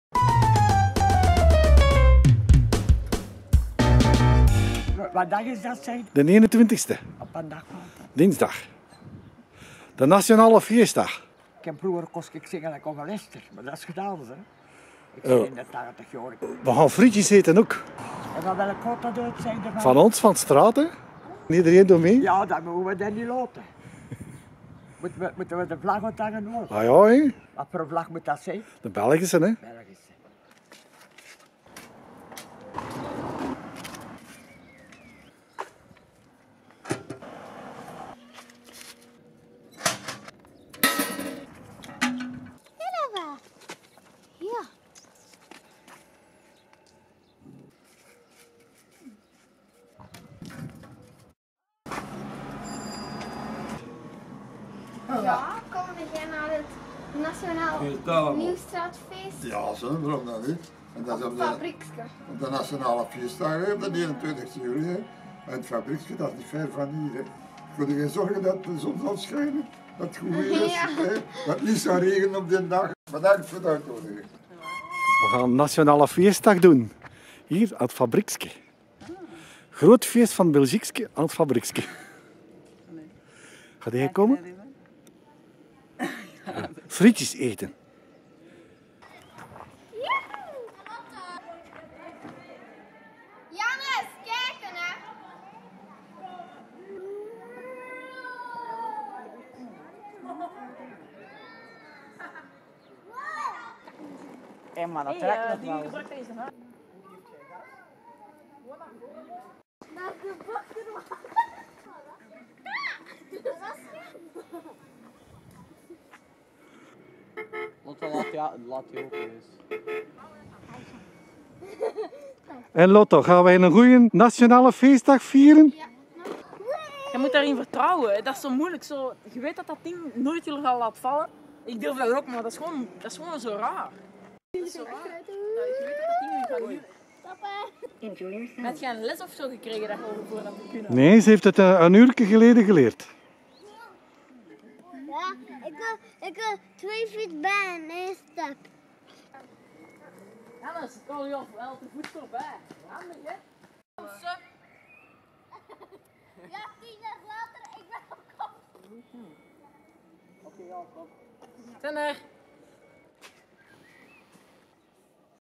20150715 voorbereiding GROOT feest van 't Belgiekske aan 't Fabriekske
op 21 juli vieren we de Nationale Feestdag in Kruiibeke!

Lekker Belgisch Bier, Heerlijke Belgische Friet, leuke speeltjes en Blue Floor treedt op!

kom langs vanaf 13 uur op het speelplein 't Fabriekske in de Nieuwstraat te Rupelmonde!